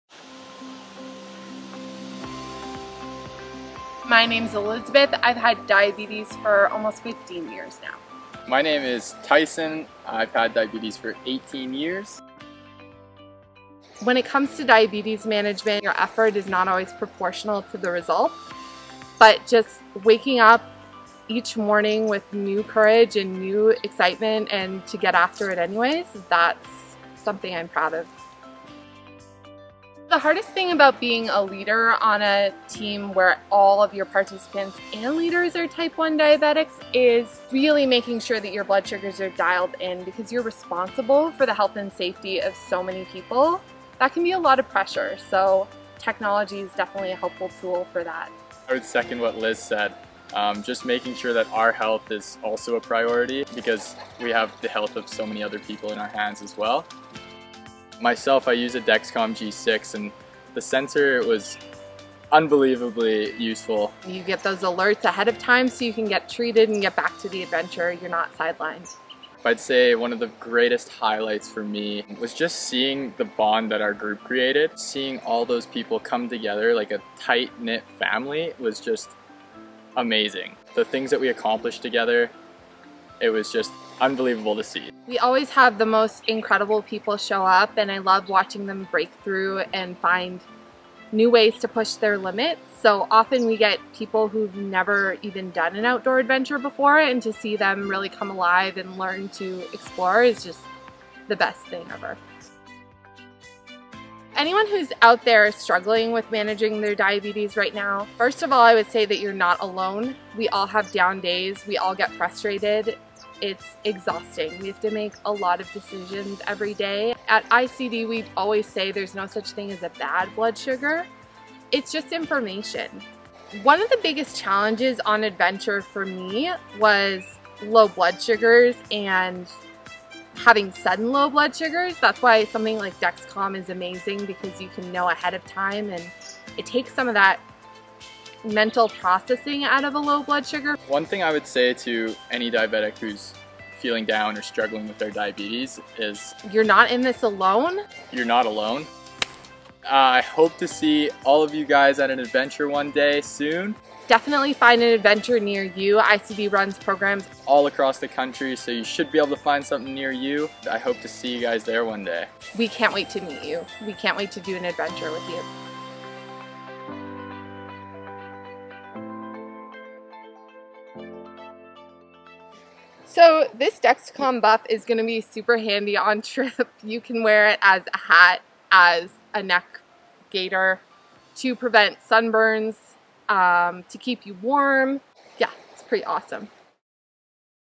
ICD Leaders Talk West Coast Adventure 2021 - Story for Success | I Challenge Diabetes
Liz and Tyson share their experiences as Adventure Team Leaders on our West Coast Adventure 2021!

Thank you to Dexcom for your support of our 2021 West Coast program.